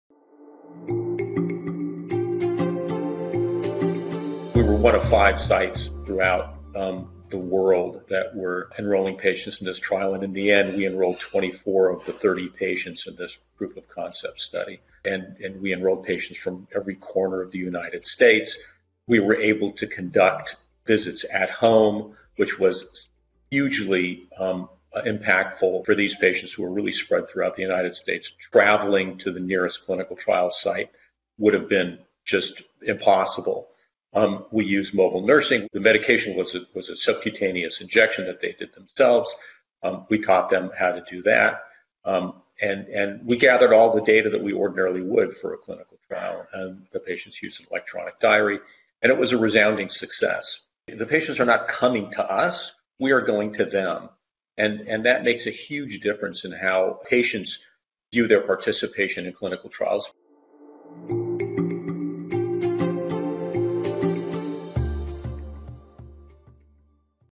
Patient Recruitment & Retention: Agile Clinical Trials in CNS Research
Dr. David Kudrow, Medical Director, Neurology at Science 37 provides insights into the success we've had with patient recruitment and retention with our agile clinical trials in cns research.

This was part of a recent webinar event, where Science 37 presented the results of a new study that looks at how future trends in clinical research will affect the CNS (central nervous system) space.